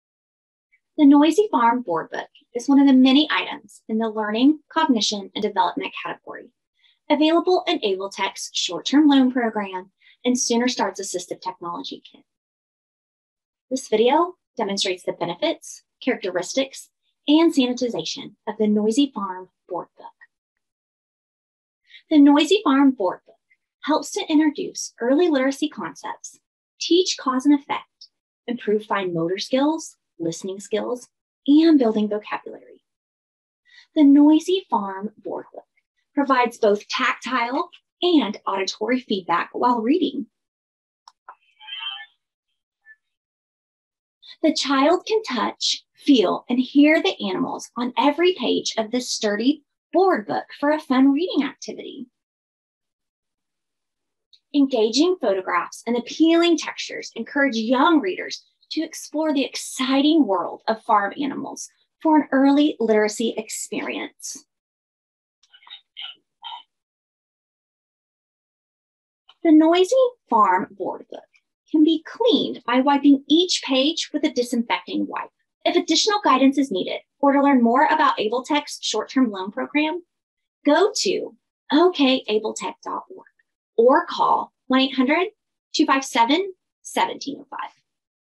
Noisy Farm Board Book
The Noisy Farm (My First) board book provides both tactile and auditory feedback while reading. The child can touch, feel, and hear the animals on every page of this sturdy board book for a fun reading activity. Engaging photographs and appealing textures encourage young readers to explore the exciting world of farm animals for an early literacy experience.